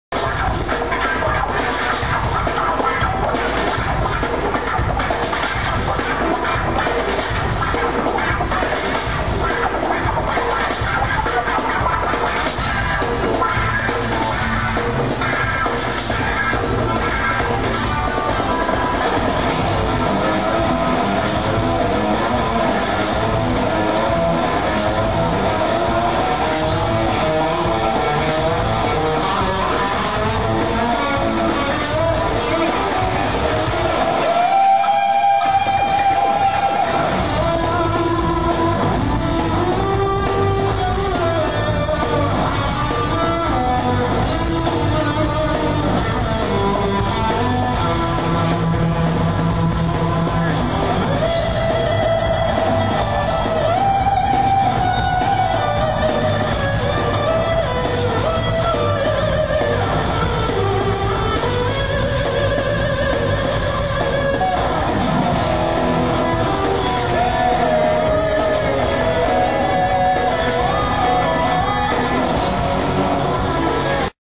Steve Vai as the Cyborg
You think ya know how to play the guitar? Watch Steve Vai kill it as a cyborg.